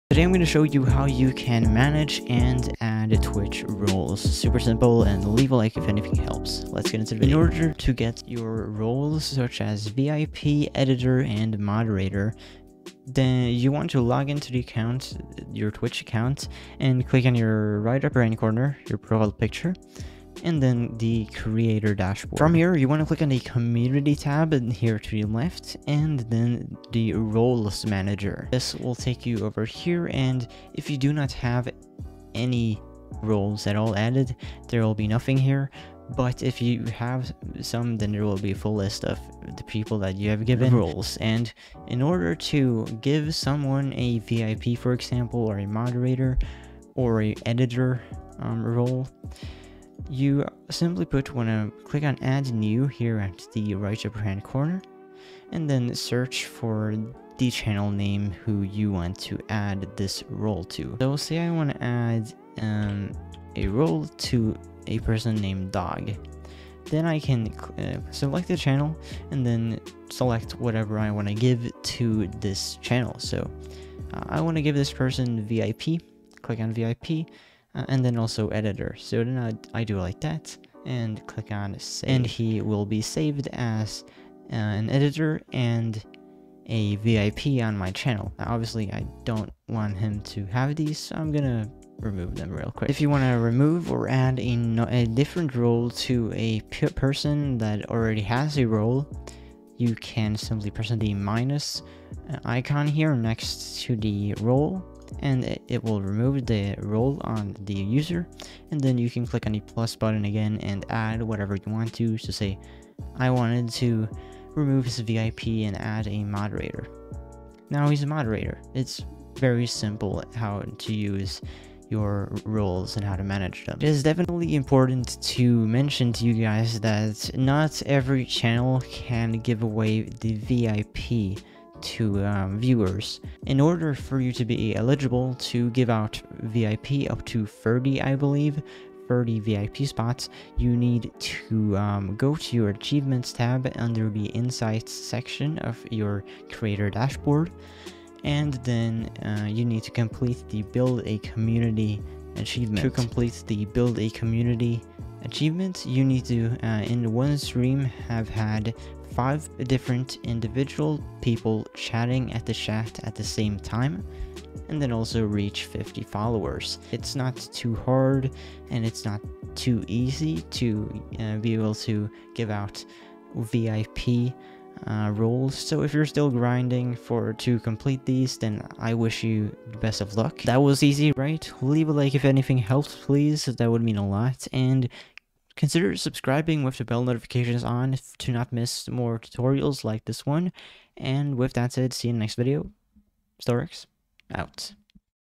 How To Manage Twitch ROLES (VIP, Moderator, Editor) - Add Twitch Roles To Livestream
Today I'm gonna go through how to manage and add Twitch roles such as VIP, Moderator and Editor to your livestreams - in only 3 MINUTES!

Grow YOUR Youtube/Twitch Channel TO THE NEXT LEVEL using Streamlabs Prime TODAY!

TIMESTAMPS:
0:00 Manage Your Twitch Roles
2:03 How To Get Access To VIP Roles

CYA!